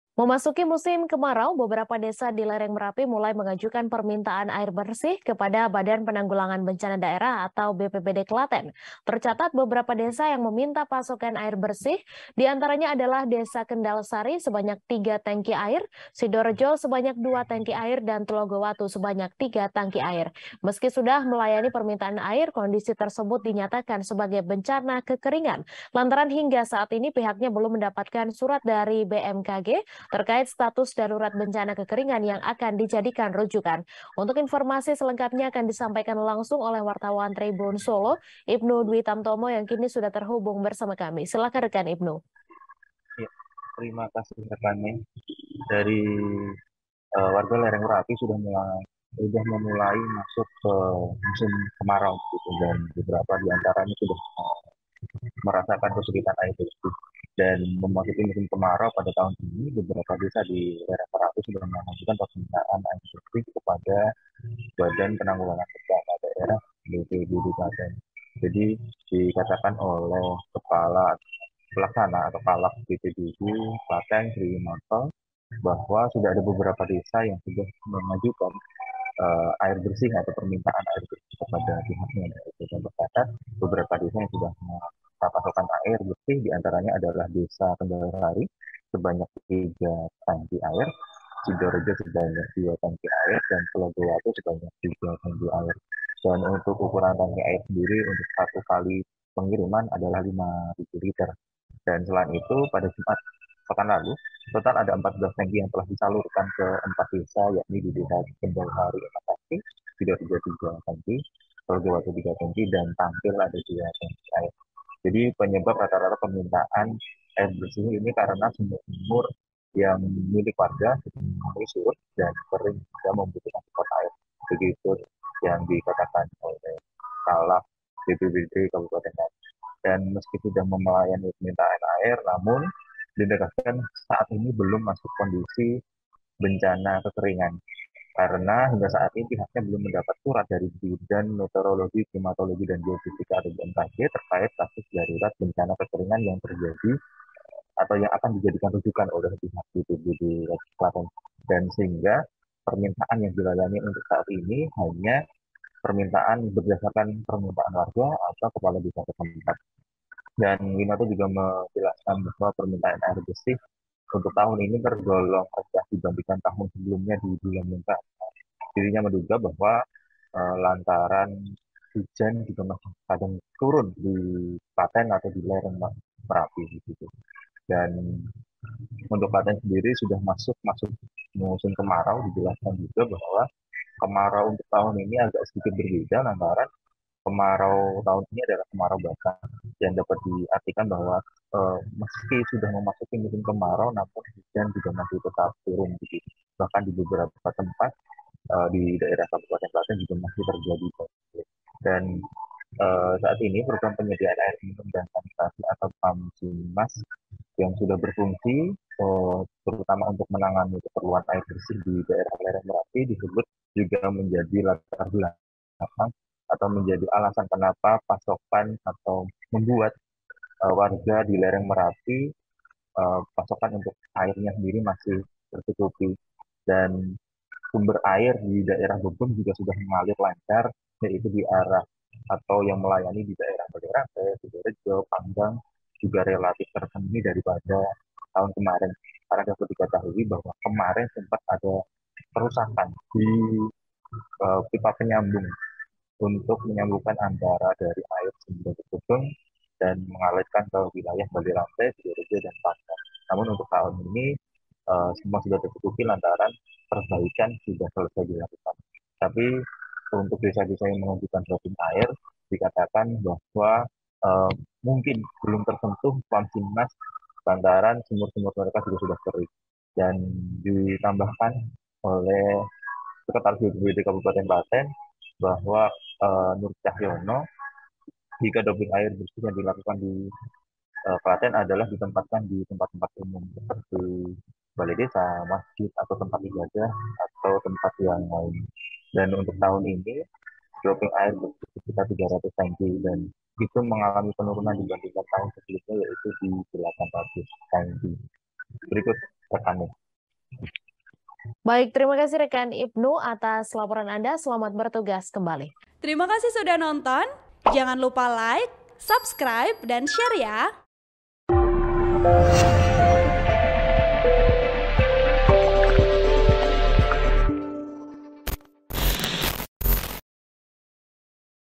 Warga Lereng Merapi di Klaten Kesulitan Air Bersih, Mulai Ajukan Permintaan Air Bersih ke BPBD
TRIBUN-VIDEO.COM - Memasuki musim kemarau, beberapa desa di lereng merapi mulai ajukan permintaan air bersih kepada Badan Penanggulangan Bencana Daerah (BPBD) Klaten.

Tercatat beberapa desa yang meminta pasokan air bersih diantaranya adalah Desa Kendalsari sebanyak tiga tangki air, Sidorejo sebanyak dua tangki air dan Tlogowatu sebanyak tiga tangki air.

Meski sudah melayani permintaan air, kondisi tersebut dinyatakan sebagai bencana kekeringan.

Lantaran, hingga saat ini pihaknya belum mendapatkan surat dari Badan Meterologi, Klimatologi dan Geofisika (BMKG) terkait status darurat bencana kekeringan yang akan dijadikan rujukan. (*)